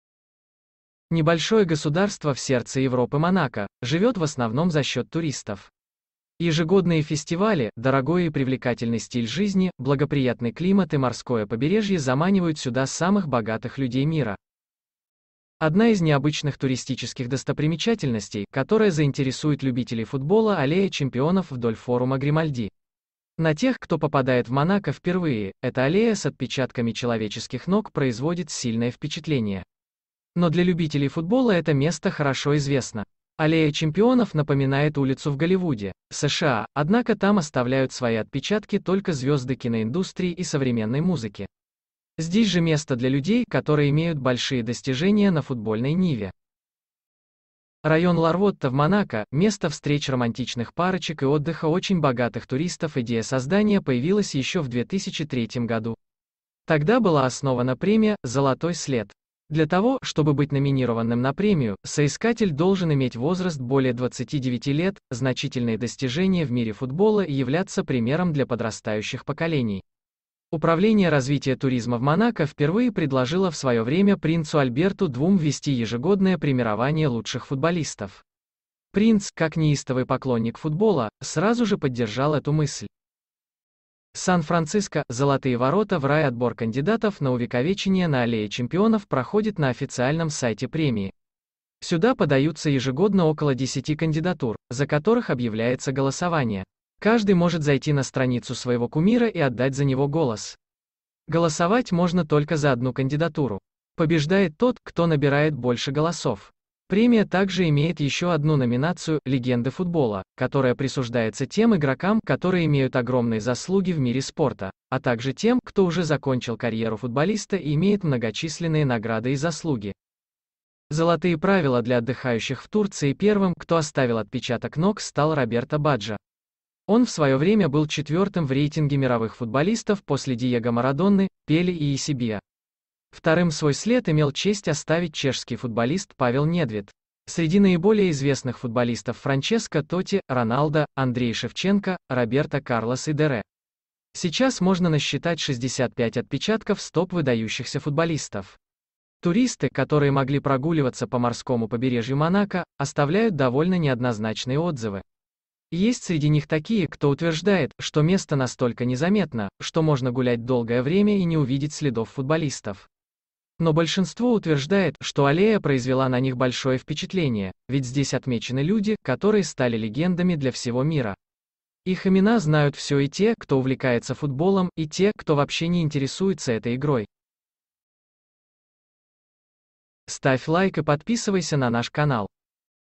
Аллея чемпионов в Монако: золотые отпечатки кумиров
Содержание:Необычная достопримечательностьКто попадает на Аллею ЧемпионовПроцедура отбора кандидатовПервые отпечаткиЧто говорят туристы Небольшое государство в сердце Европы Монако, живет в основном за счет туристов. Ежегодные фестивали, дорогой и привлекательный стиль жизни, благоприятный климат и морское побережье заманивают сюда самых богатых людей мира. Необычная достопримечательность Одна из необычных туристических достопримечательностей, которая заинтересует любителей футбола Аллея []